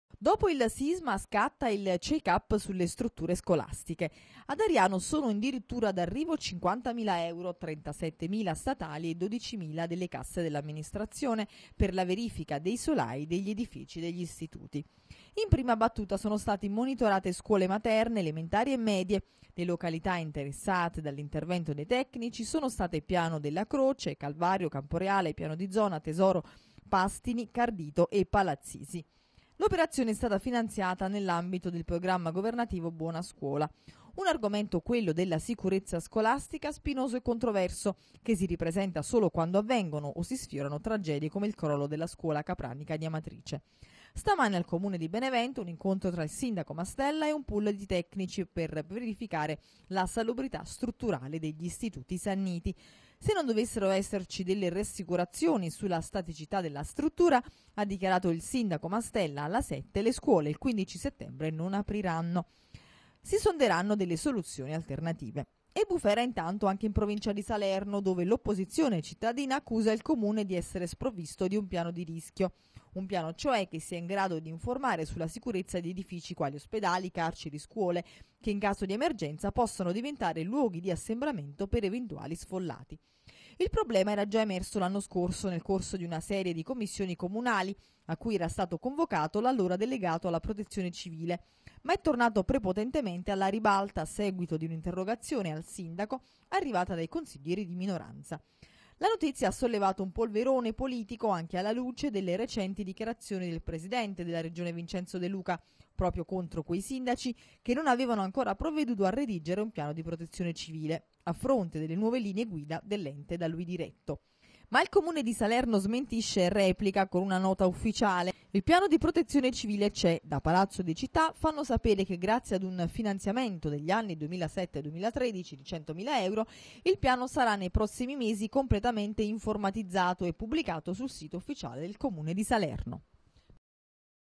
SCUOLE E SICUREZZA CHECK-UP IN IRPINIA BUFERA A SALERNO
Mentre ad Ariano Irpino si ultima il check-up sulle scuole, nel Sannio  i primi controlli. A Salerno scoppia la bufera.